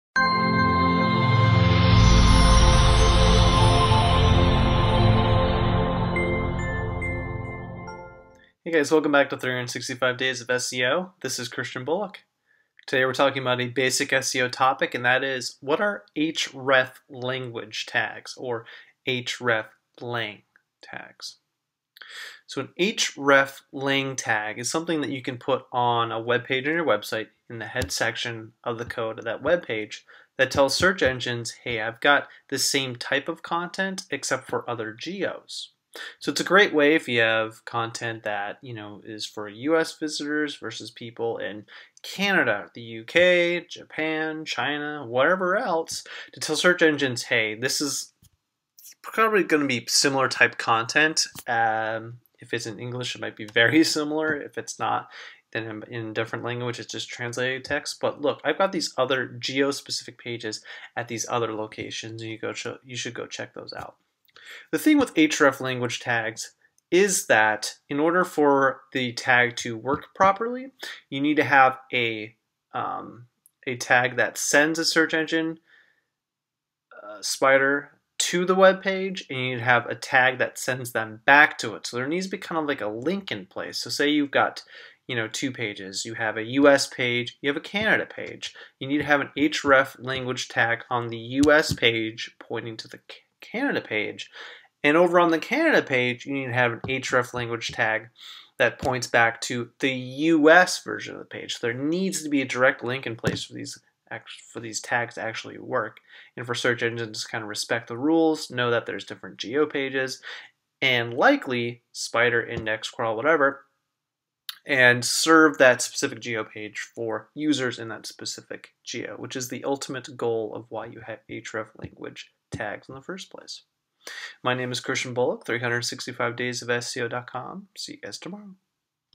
Day #166: What are hreflang tags (and how to use them for SEO)
In today's video I talk about what hreflang tags are and how to utilize them for SEO purposes.